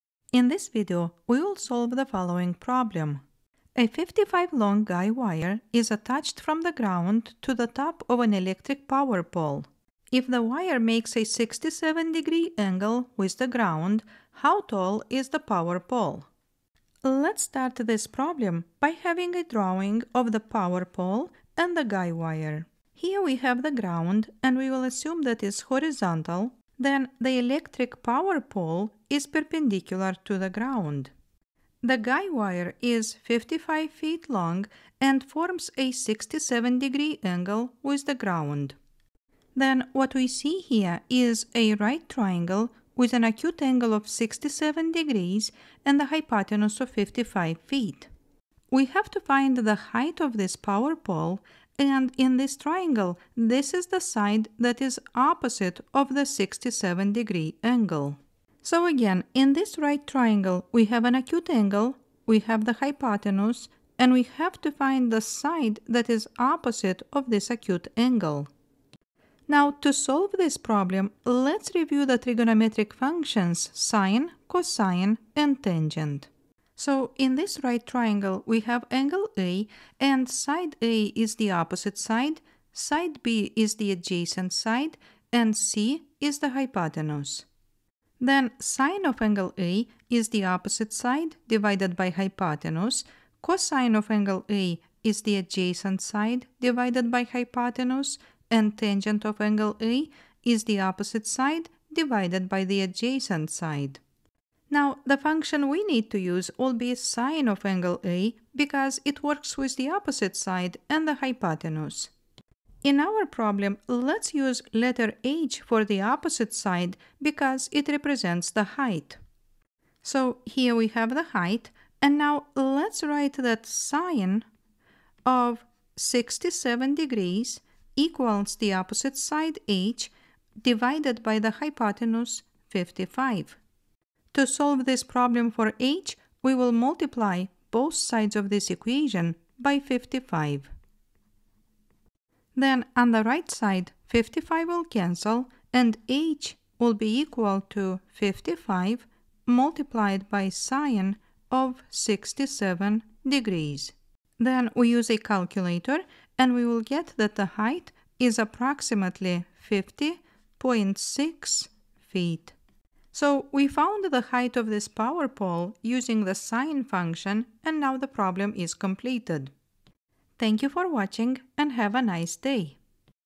Word Problem: Find the Height of the Power Pole
A 55 feet long guy wire is attached from the ground to the top of an electric power pole. If the wire makes a 67° angle with the ground, how tall is the power pole?